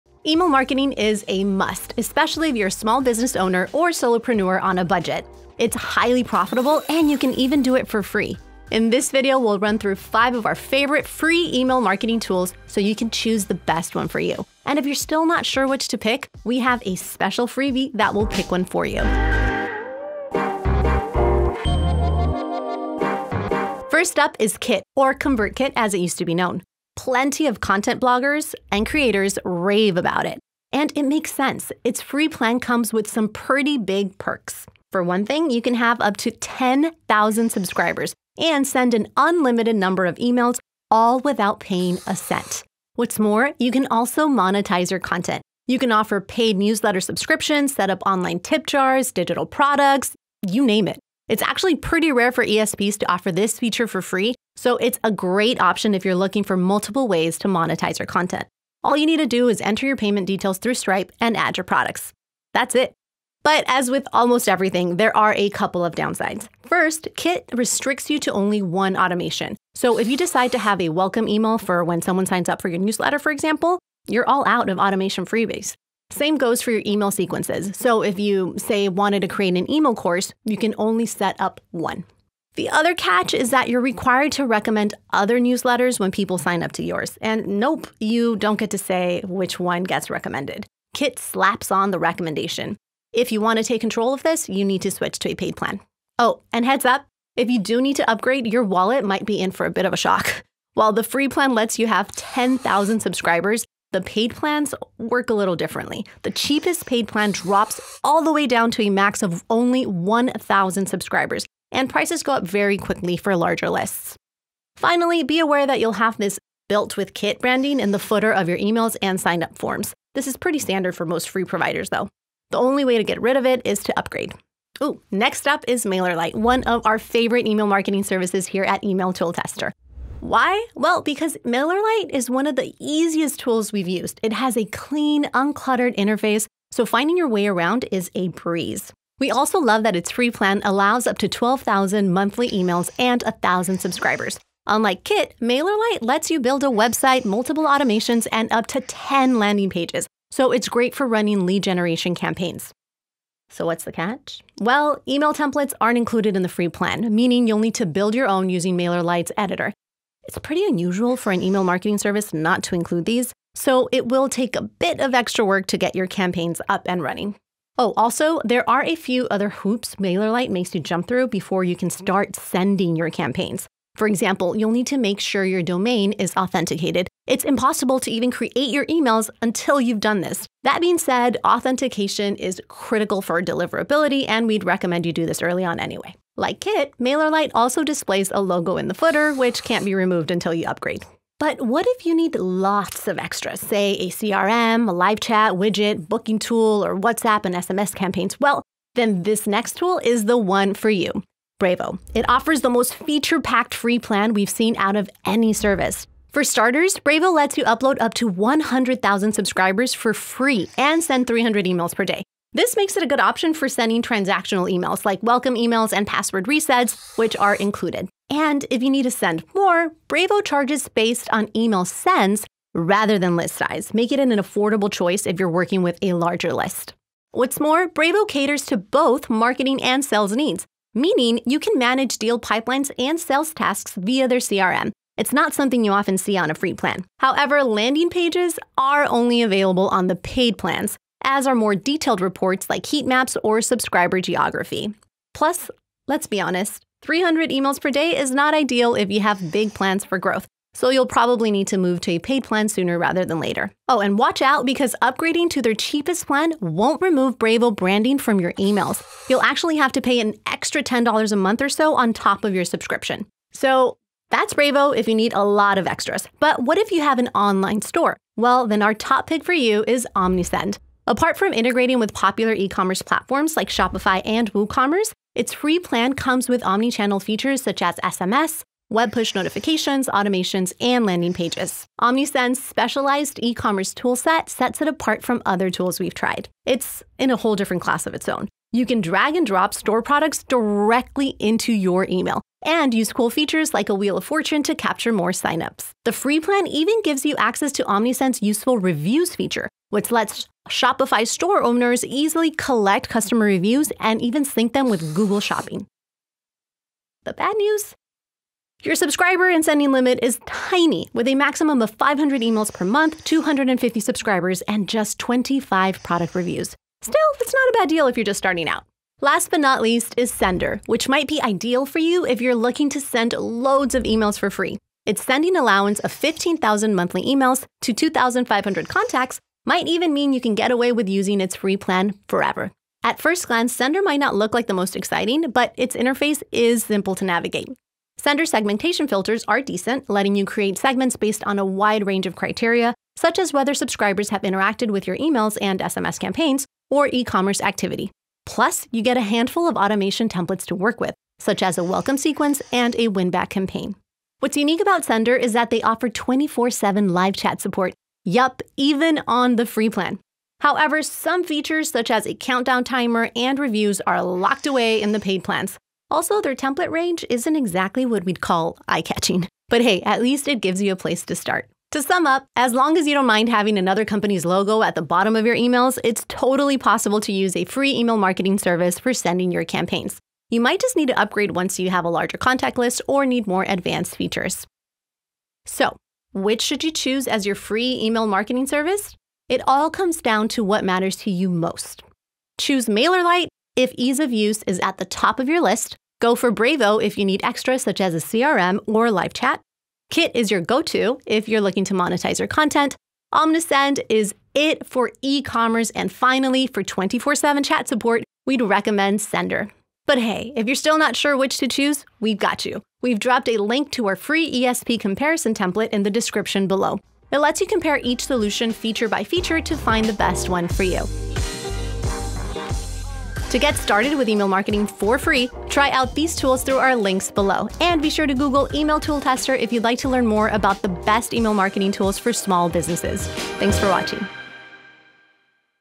Best Free Email Marketing Software in 2026: What's the Catch? (Brevo, MailerLite, Omnisend + ??)
One of the best things about email marketing is its high return on investment. In fact, you don't need to spend a cent to get started!

In this video, we'll take you through the top 5 free email marketing tools for small businesses, solopreneurs, and content creators. We'll explain the pros, the cons, and what limitations you might come up against in their free plans.

Having trouble picking the right one? Download our free email marketing software comparison template

Contents:
0:00 Intro
0:23 Kit
2:05 MailerLite
3:11 Brevo
4:26 Omnisend
5:19 Sender
6:11 Conclusion